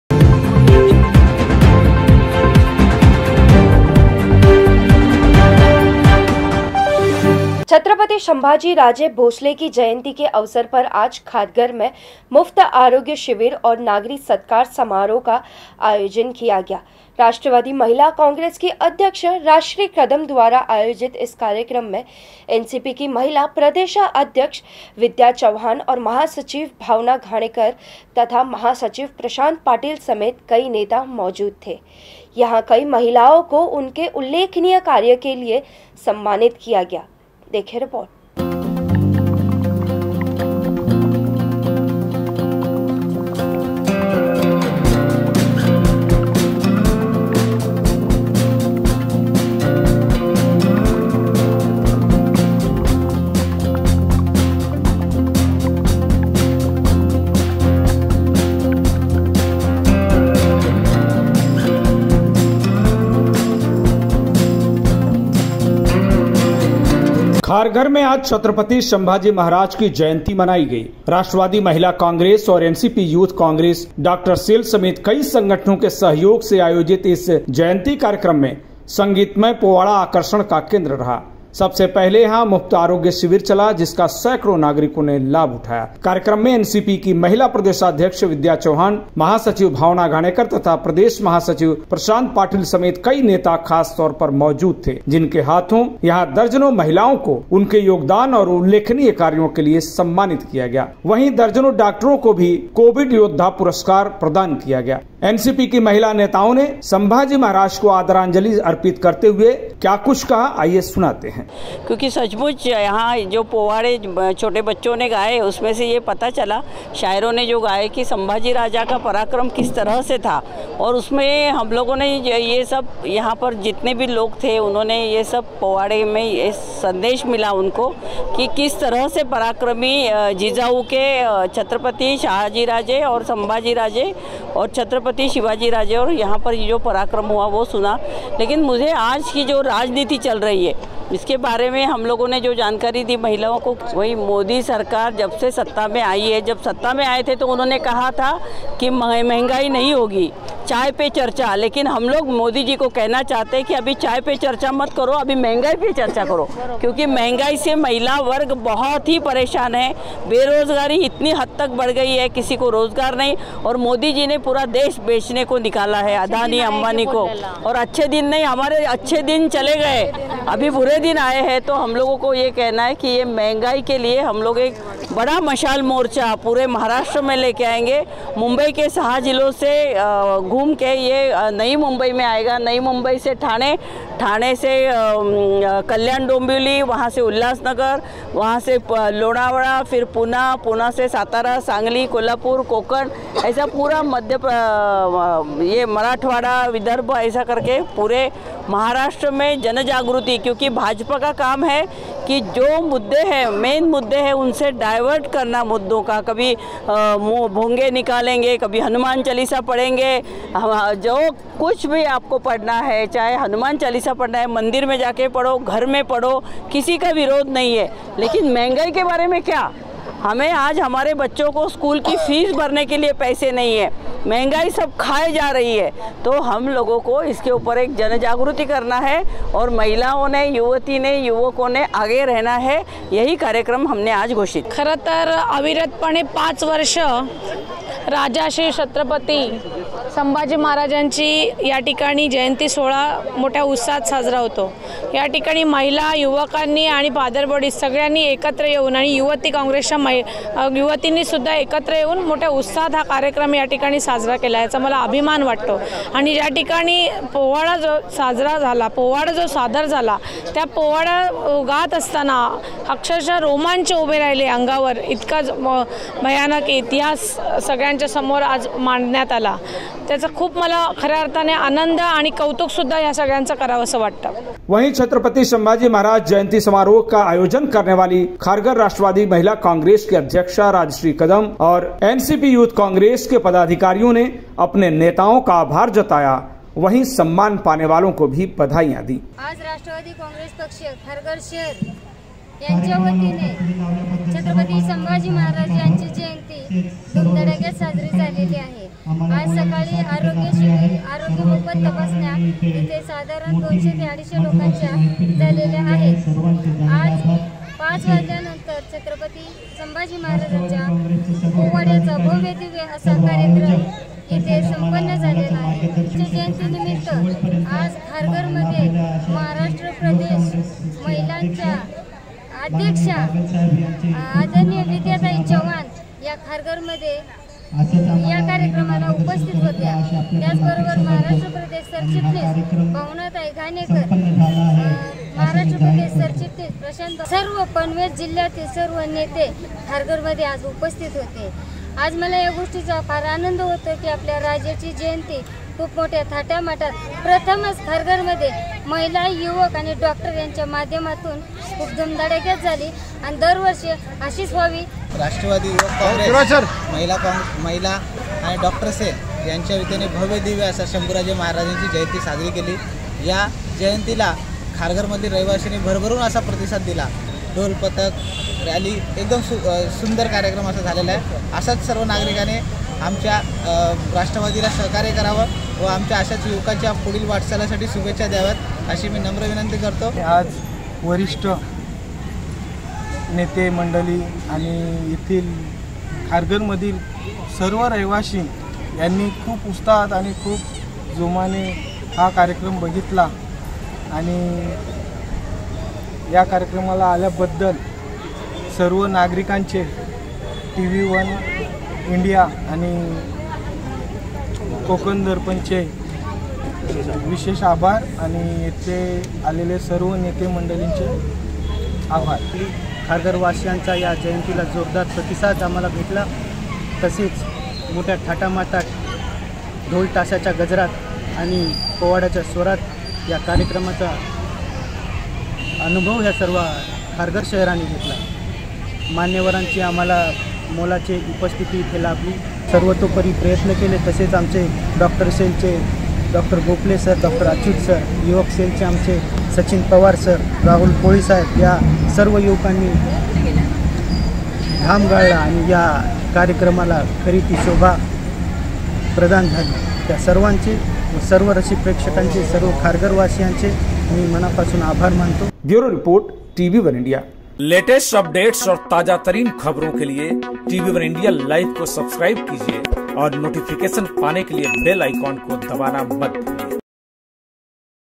Rajshree Kadam | NCP महिला कांग्रेस ने किया महिलाओं को सम्मानित | SambhaJi Maharaj Jayanti
छत्रपति संभाजीराजे भोसले की जयंती के अवसर पर आज खारघर में मुफ्त आरोग्य शिविर और नागरी सत्कार समारोह का आय़ोजन किया गया. राष्ट्रवादी महिला कांग्रेस की अध्यक्षा राजश्री कदम द्वारा आयोजित इस कार्यक्रम में एनसीपी की महिला प्रदेशाध्यक्ष विद्या चव्हाण और महासचिव भावना घाणेकर तथा महासचिव प्रशांत पाटिल समेत कई नेता मौजूद थे. यहां कई महिलाओं को उनके उल्लेखनीय कार्यों के लिए सम्मानित किया गया